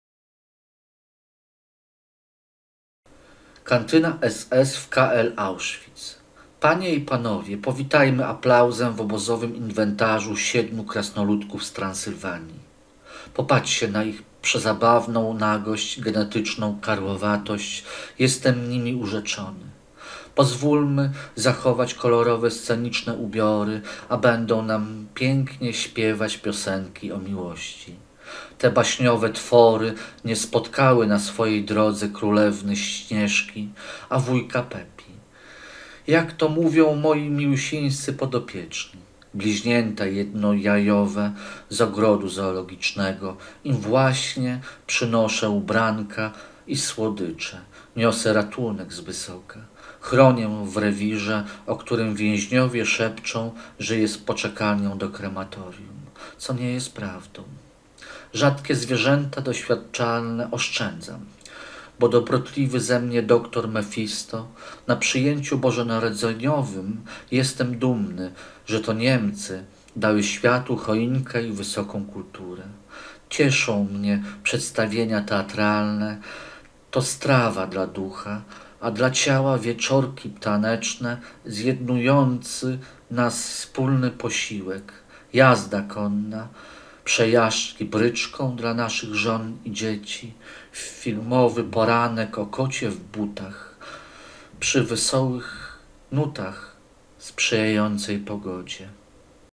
Kantyna SS w KL Auschwitz
Panie i Panowie powitajmy aplauzem w obozowym inwentarzu
siedmiu krasnoludków z Transylwanii
popatrzcie na ich przezabawną nagość genetyczną karłowatość
jestem nimi urzeczony!
pozwólmy zachować kolorowe sceniczne ubiory
a będą nam pięknie śpiewać piosenki o miłości

Te baśniowe twory nie spotkały na swojej drodze
królewny Śnieżki

a Wujka Pepi jak to mówią moi milusińscy podopieczni
bliźnięta jednojajowe z ogrodu zoologicznego
im właśnie przynoszę ubranka i słodycze niosę ratunek z wysoka
chronię w rewirze o którym więźniowie szepczą
że jest „poczekalnią do krematorium”
co nie jest prawdą

Rzadkie zwierzęta doświadczalne
oszczędzam bo dobrotliwy ze mnie doktor Mefisto
na przyjęciu bożonarodzeniowym
Jestem dumny że to Niemcy dały światu choinkę i wysoką kulturę
cieszą mnie przedstawienia teatralne
to strawa dla ducha a dla ciała wieczorki taneczne
zjednujący nas wspólny posiłek jazda konna
przejażdżki bryczką dla naszych żon i dzieci
filmowy poranek ‘O kocie w butach’
przy wesołych nutach sprzyjającej pogodzie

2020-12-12